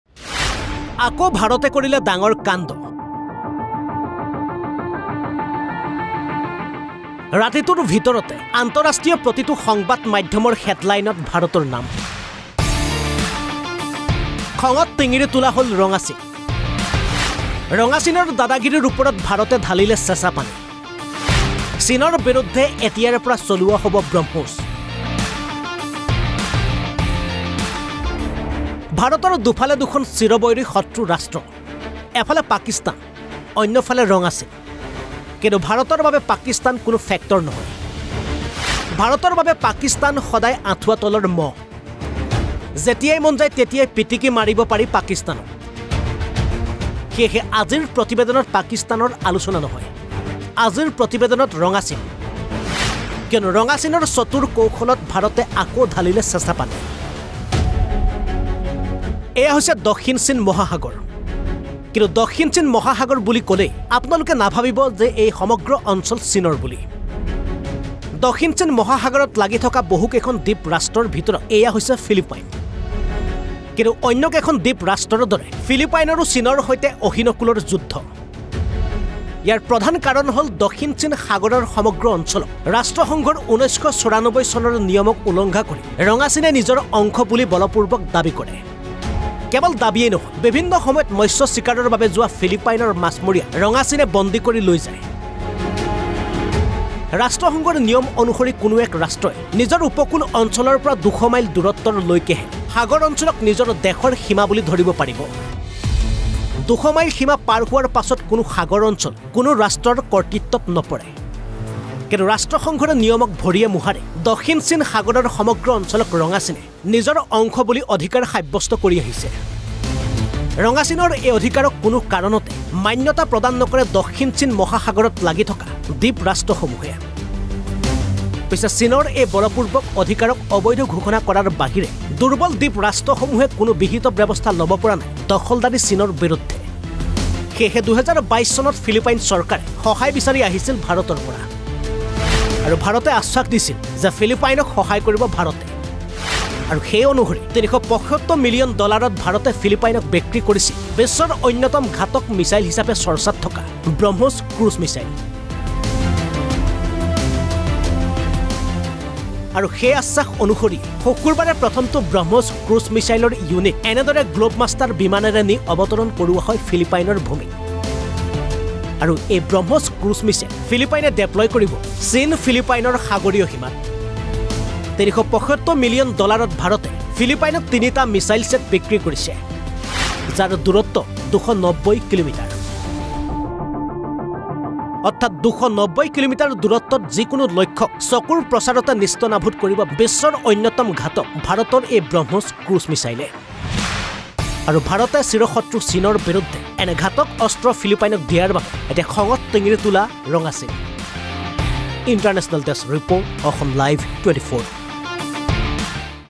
ৰঙা চীনক ভাৰতৰ চুঙা চাই সোপা।
ৰঙা চীনক ভাৰতৰ চুঙা চাই সোপা। সামান্য খেলিমেলি কৰিলেই চীনৰ বিৰুদ্ধে চলোৱা হ'ব ব্ৰক্ষ্মছ মিছাইল।